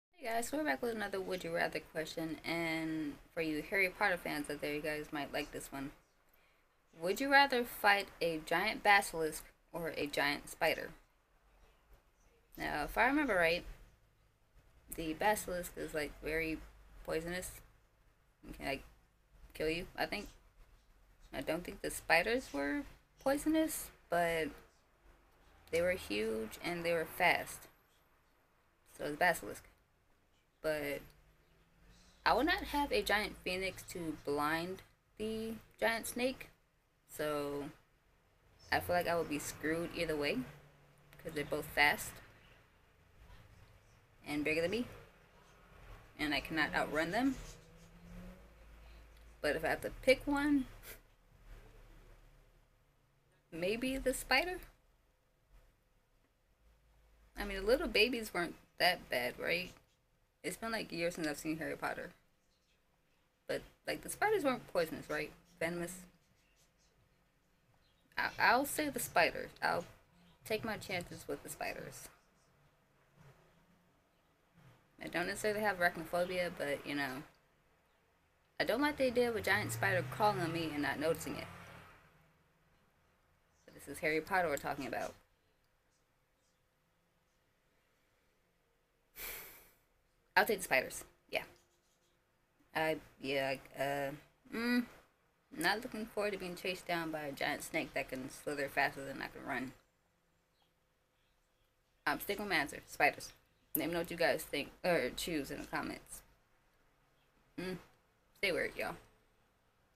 Would You Rather...?
Okay Potterheads and non Potterheads, which would you go up against? A giant spider or a giant basilisk.. Both sounds pretty bad lol but I choose the spider.

My Instagram - annienorthman89 (follow if you like, I post a bunch of random stuff)